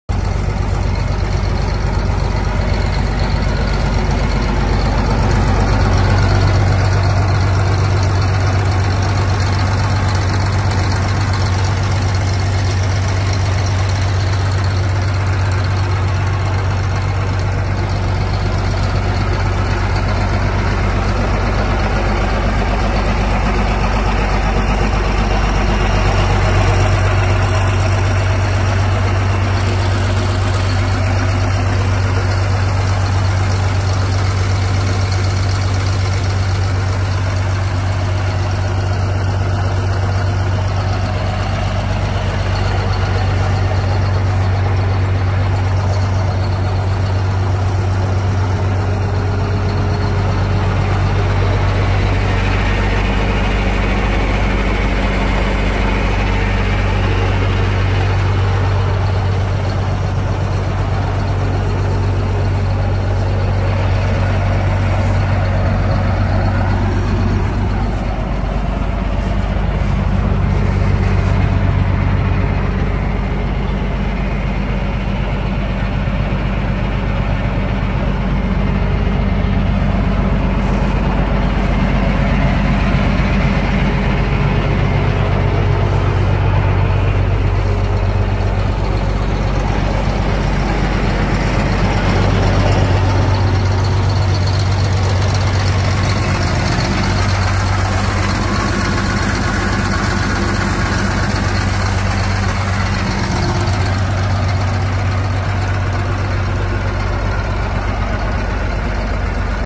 1981 ALLIS-CHALMERS 7580 For Sale
1981 Allis Chalmers 7580 Tractor
•Repainted and engine overhaul by previous owner.
•Rebuilt Bosch injectors less than 100hrs ago
•4 to 5 inch Chrome Stack
•LED lights from Tiger lights on rear and some on the front.
•LED Flashers on front
•Runs and Drives good
•Three rear remotes
•A/C works
•Three-point
•Large 1000 PTO
•24.5x32 Tires
We have used this tractor on our 700 bushel grain cart for the last 4 years and it has worked well just upgraded to an 8550.
Located Near Avon SD
We also have a 7000 Allis selling the same day.